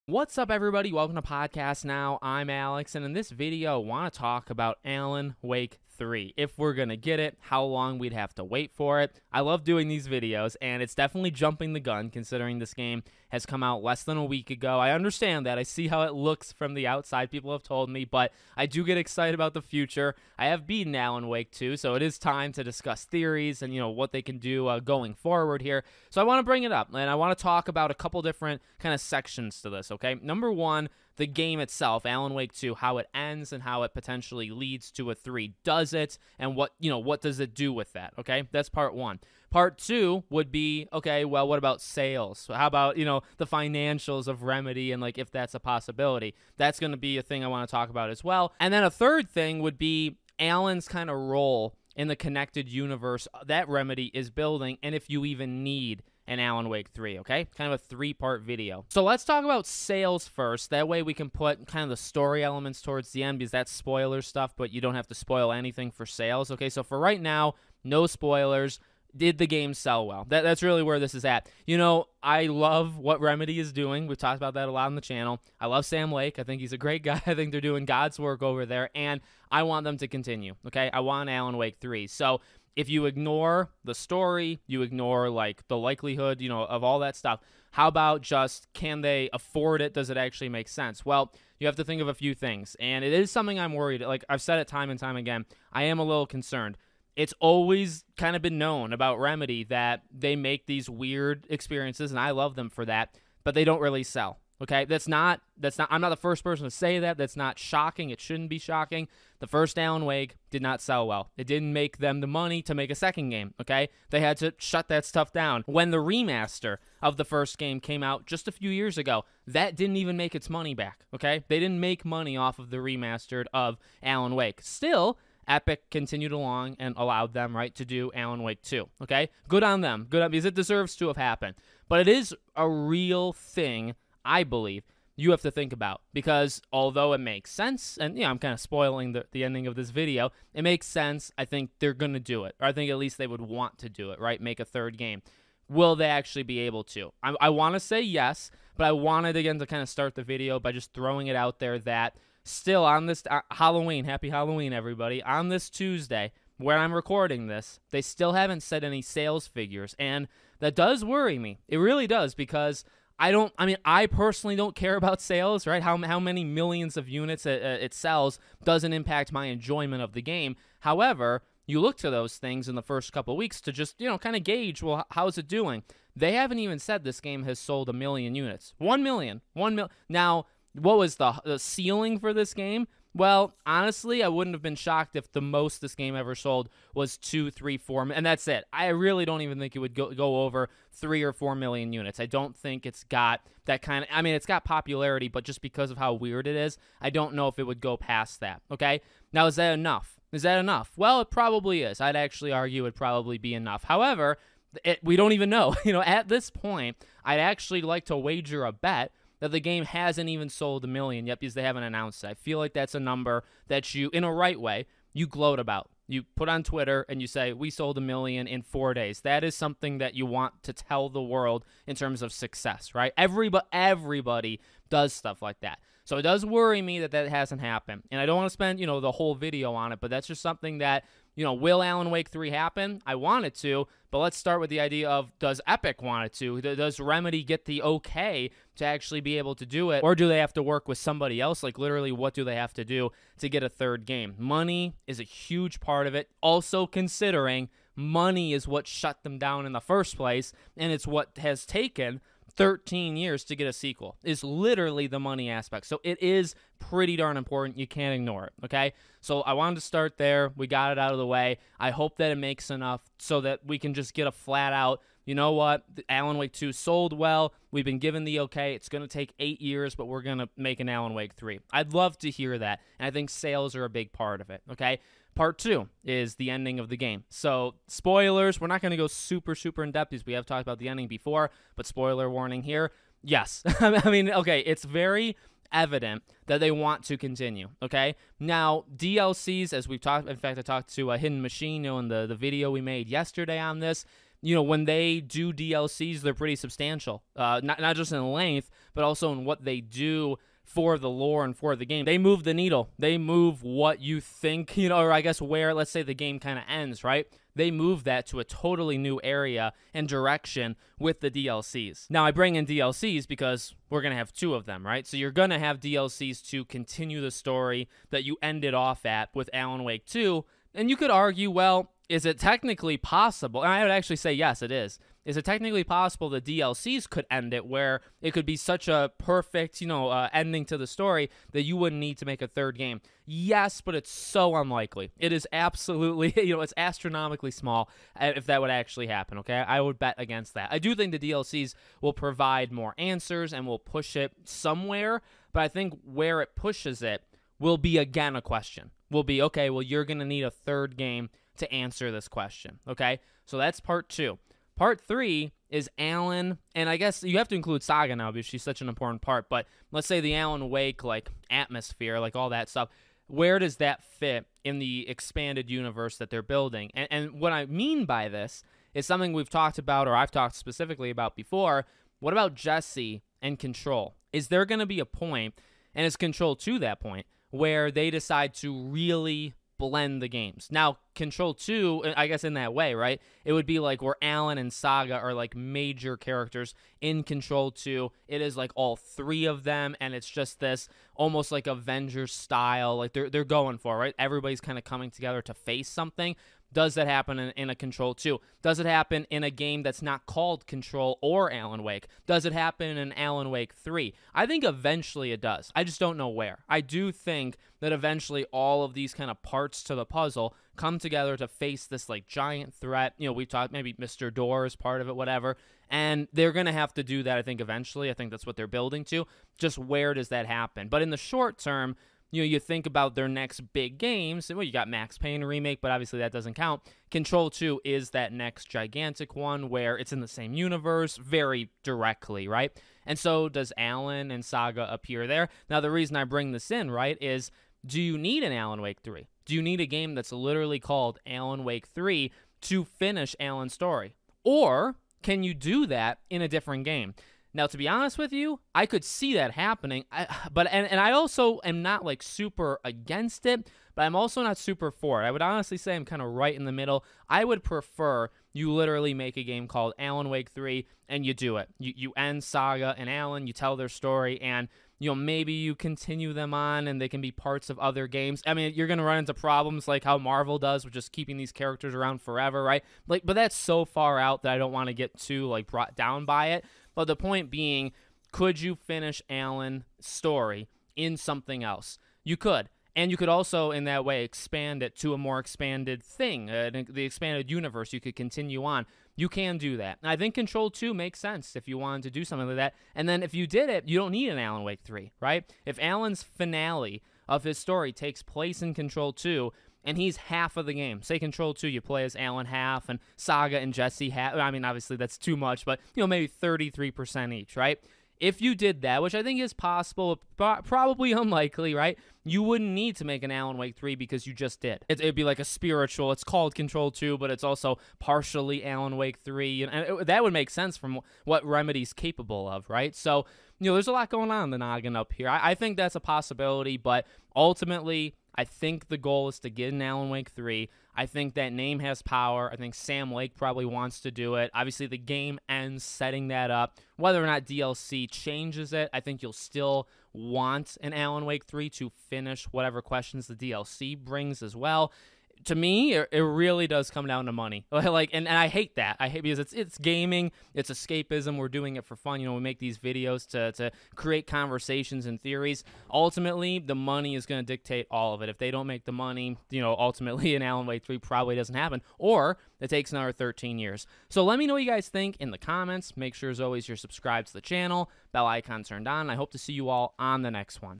Will Alan Wake 3 Happen??
Let's talk about Alan Wake 3 and the odds of it happening. From the money behind it, to if the story actually demands it happens.